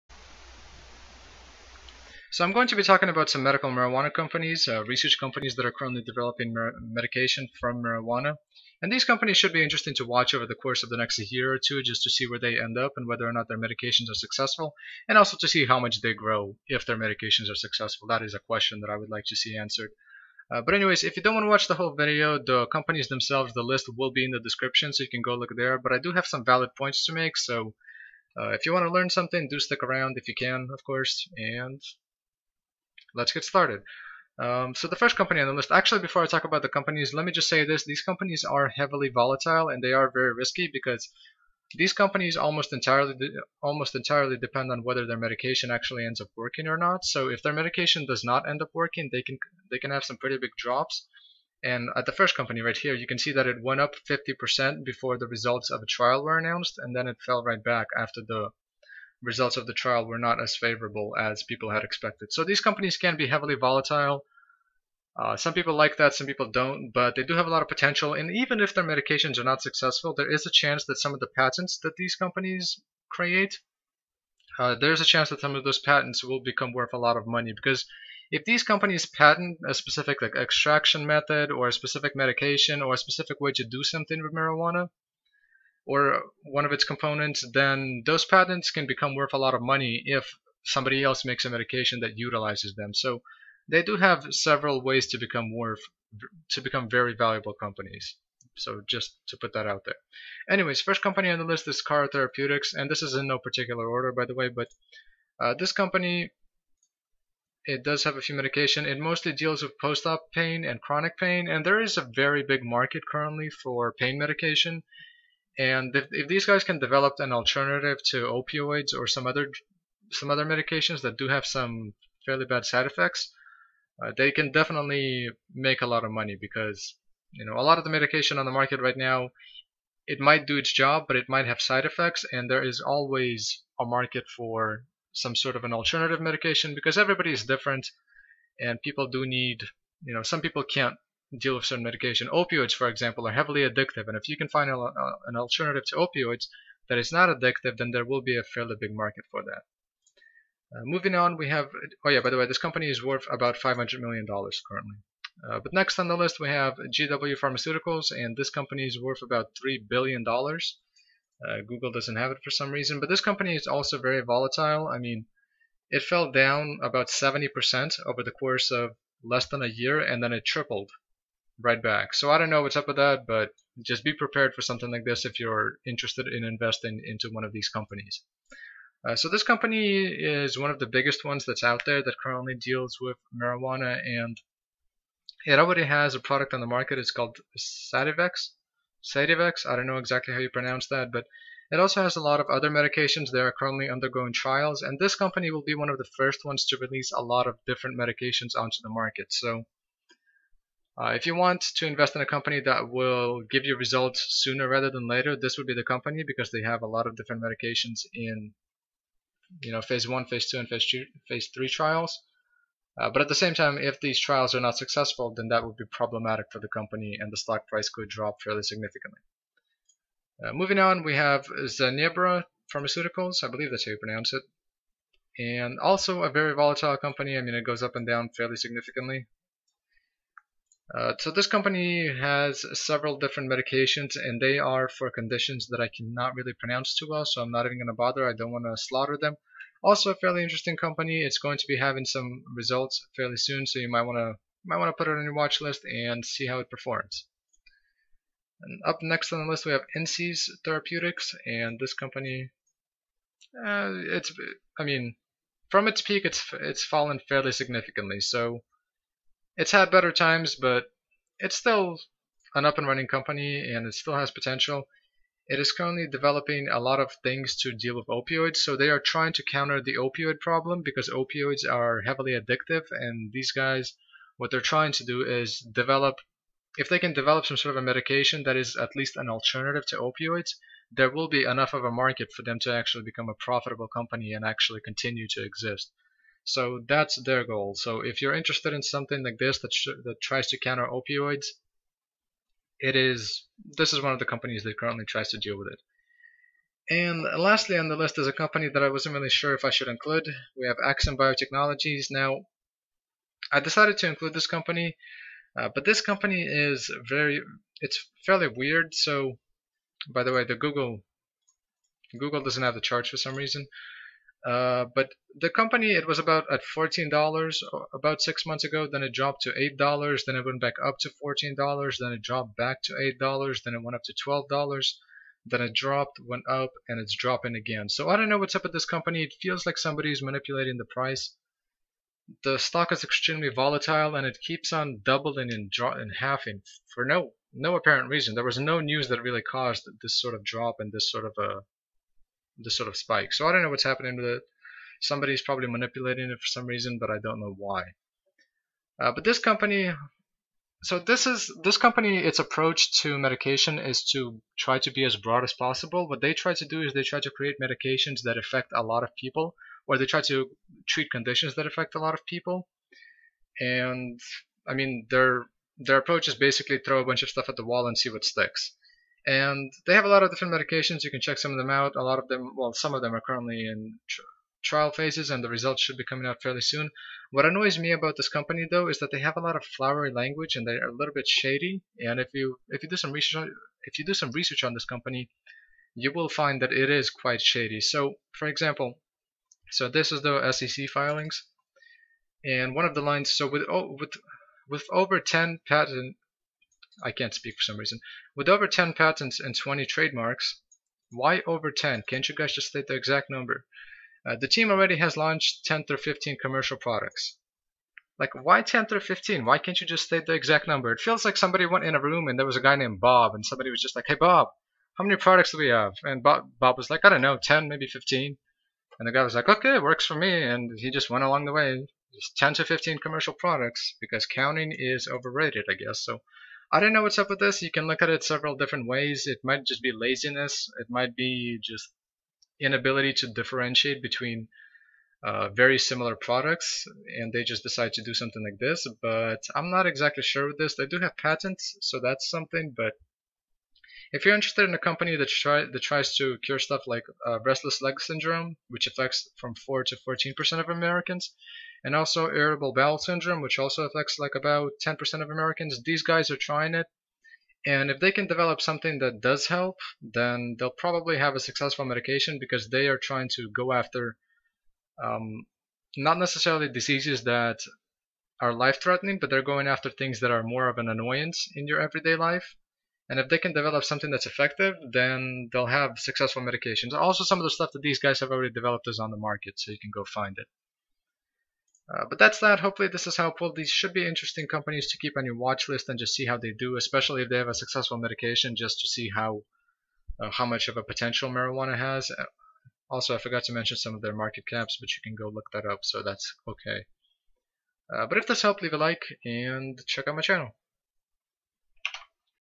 5 Medical Marijuana Research Stocks To Watch 2017 & 2018
A good read for people who are new to investing
5 cannabis research companies that are developing medications and will be releasing the trial results for those medication in 2017 and 2018. Definitely an interesting watch to see what happens and how profitable they can become. If any of these companies are successful, they will likely get a lot of media attention which will result in the stocks of these companies doing quite well. I will be covering these sorts of stocks consistently on my channel in the future. Once they are closer to reaching results and actually having financials to look at, I will cover them in more depth.

In the same order as in the video:
CARA Cara Therapeutics Inc
GWPH GW Pharmaceuticals PLC- ADR
ZYNE Zynerba Pharmaceuticals Inc
INSY Insys Therapeutics Inc
AXIM Axim Biotechnologies Inc